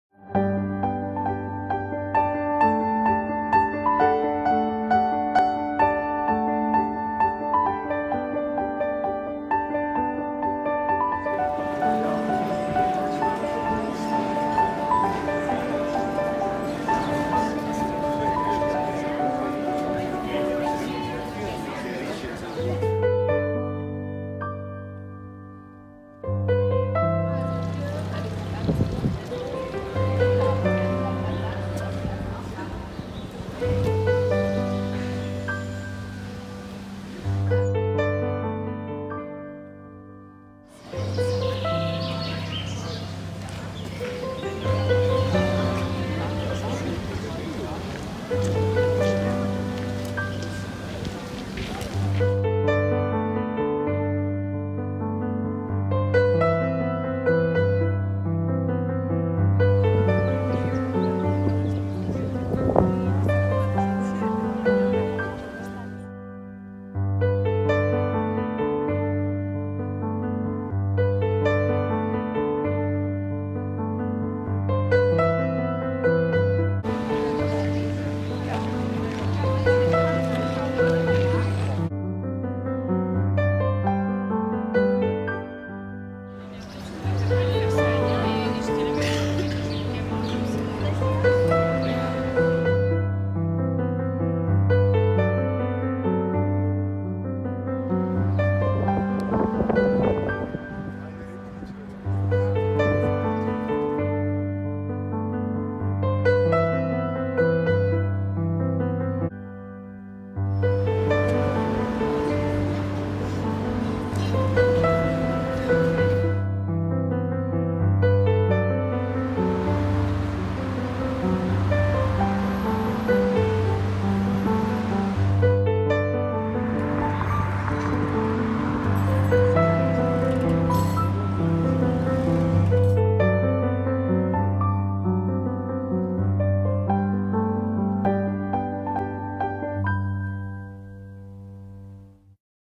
Heidelberg: Famous tourist destinations in Germany 🇩🇪
Discovering Heidelberg Castle, Old Bridge, Königstuhl, Heidelberg Palace, Haupstrasse & Old town